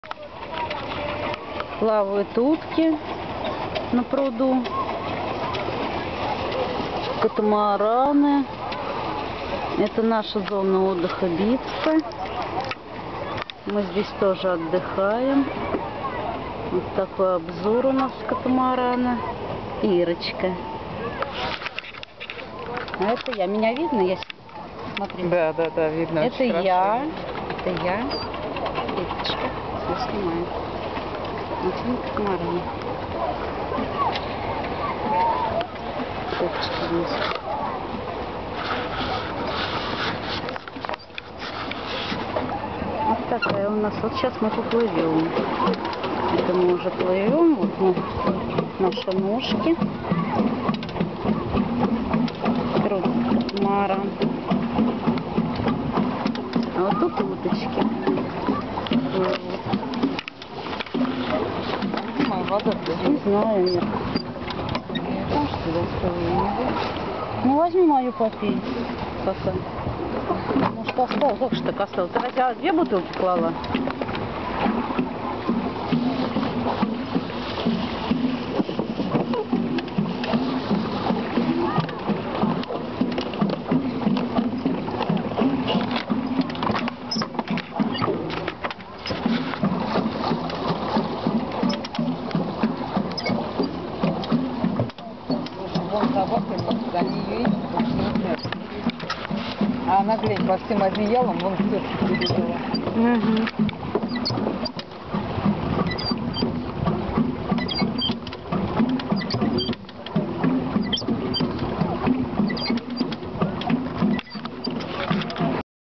Catamaran in recreation Bitsa Moscow.На катамаране в зоне отдыха Битца.
Лето в Москве; L'été a Moscou. The summer in Moscow. 2008